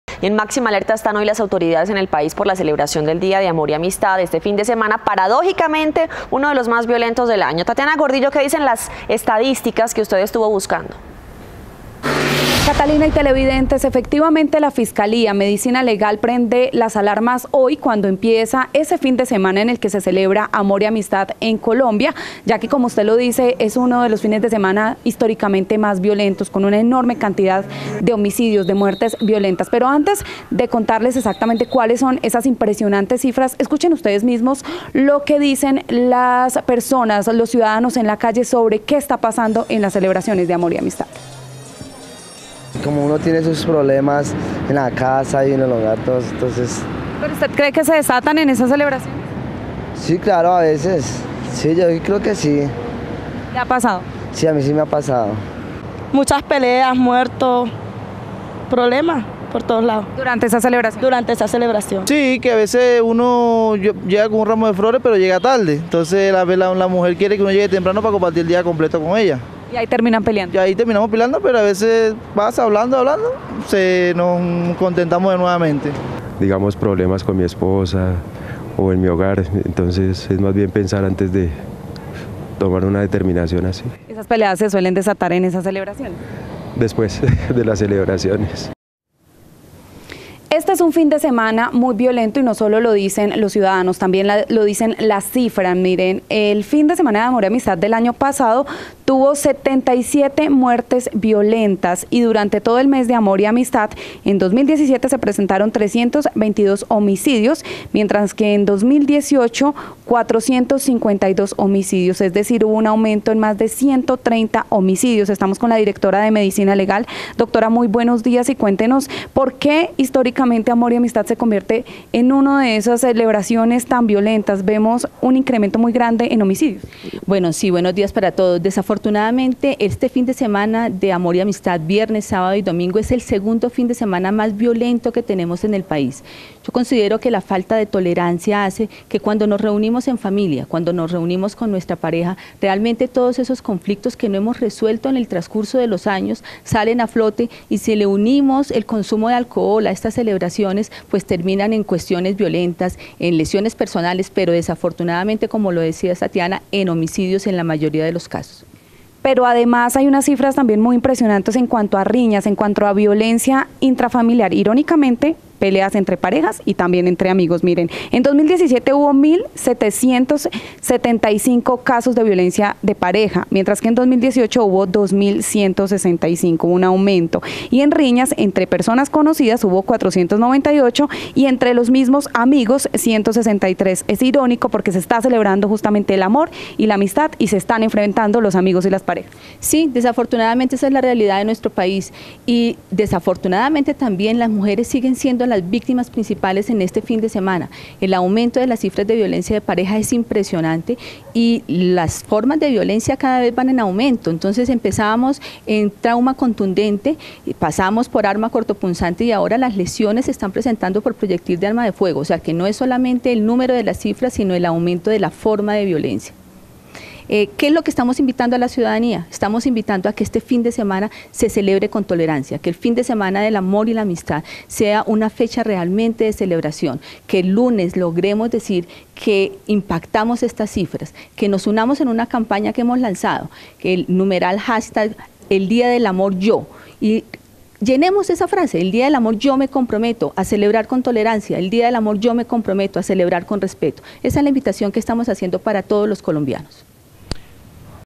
Día de amor y amistad, fecha de peleas y muertes en Colombia
Es el segundo fin de semana más sangriento del año, reveló Medicina Legal. El año pasado, durante esos días, se registraron 77 homicidios.